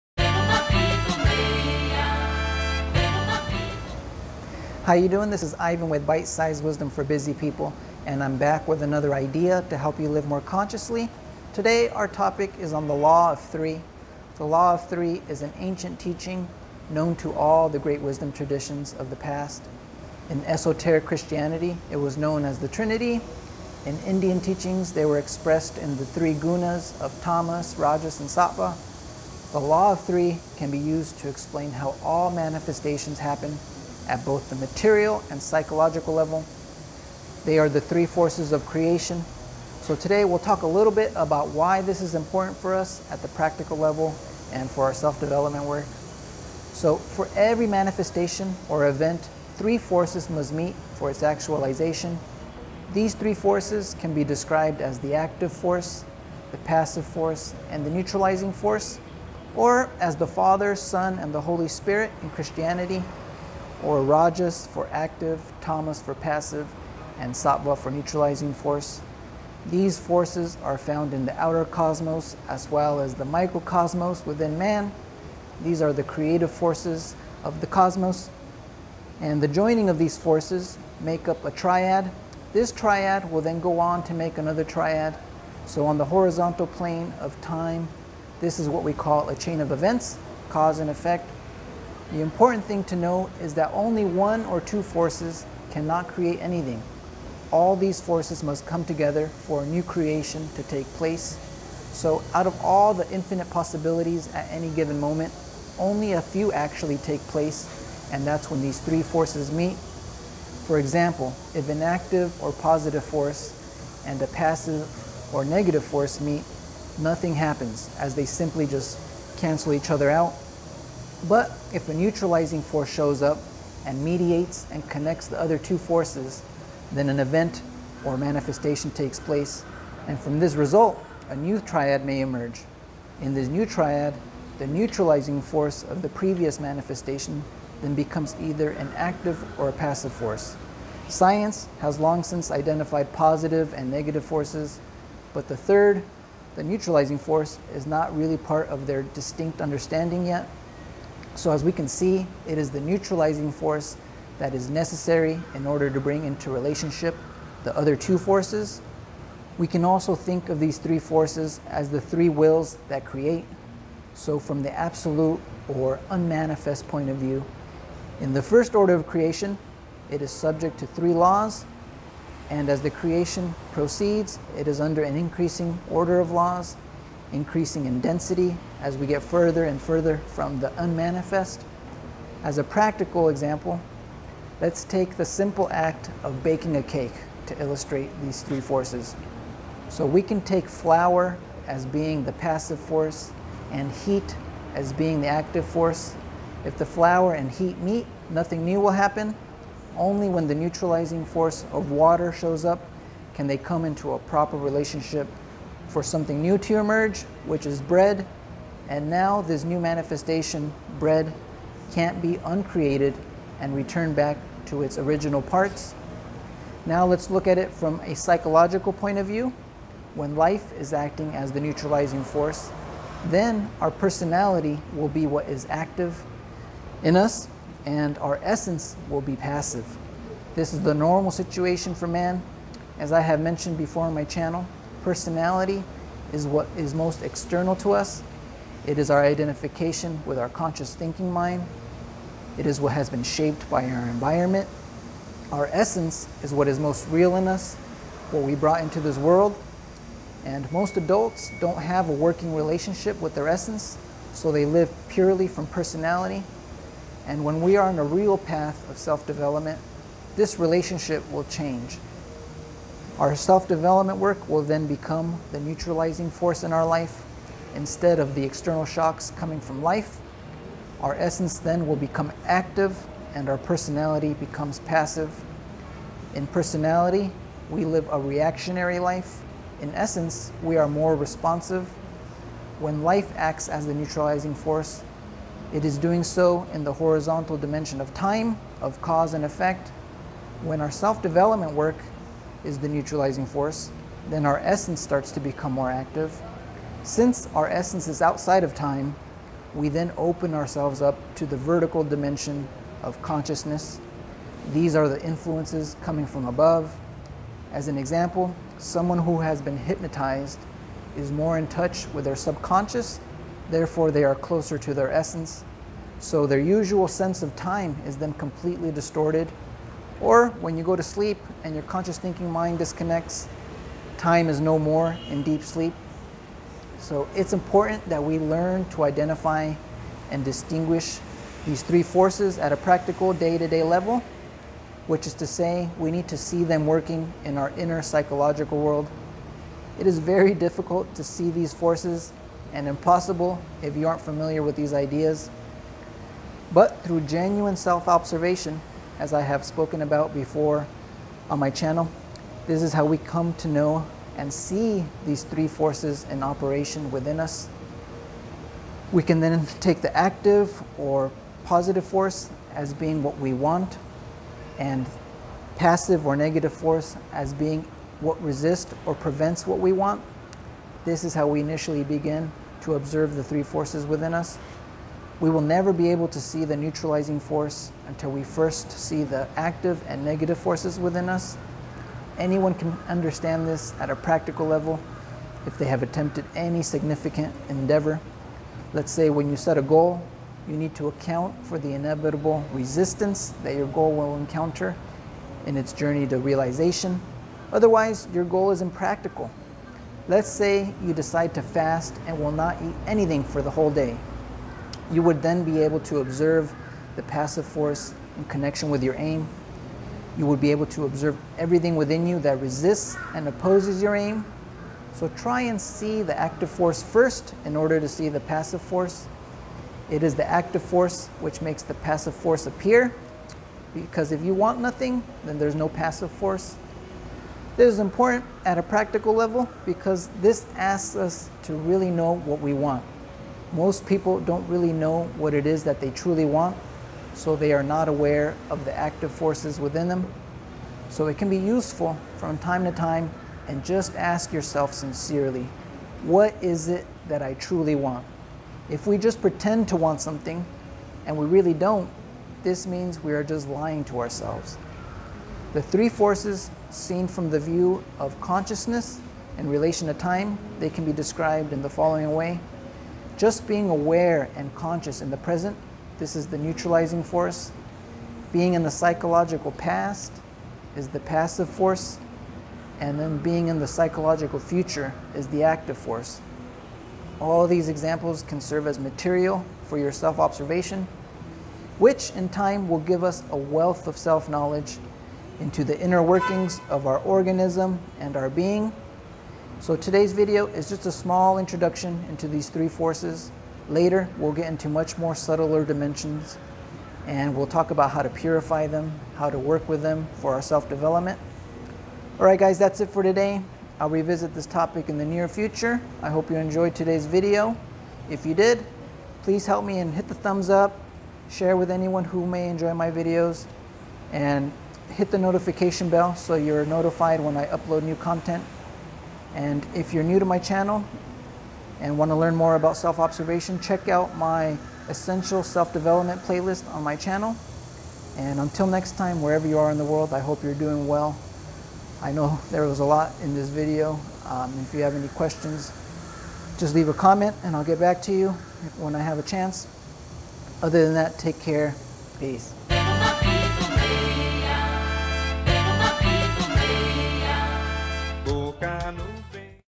The Law of Three: The 3 Forces of Manifestation (Bitesize Wisdom #134)
The law of 3 is an ancient teaching known to all the great wisdom traditions of the past. In esoteric Christianity is was known as the trinity, in Indian teachings, they were expressed in the 3 Gunas of tamas, rajas, and sattva.

The law of three can be used to explain how all manifestations happen at both the material and psychological level. It is the 3 forces of creation. We will talk a little bit about why this important for us at the practical level.

So for every manifestation or event 3 forces must meet for its actualization.

These 3 forces can be described as the active force, passive force, and neutralizing force. Or as the father, son, and the holy spirit in Christianity or Rajas for active, Tamas for passive, and Satva for neutralizing force. These forces are found in the outer cosmos as well as the microcosmos within man. These are the creative forces of our cosmos.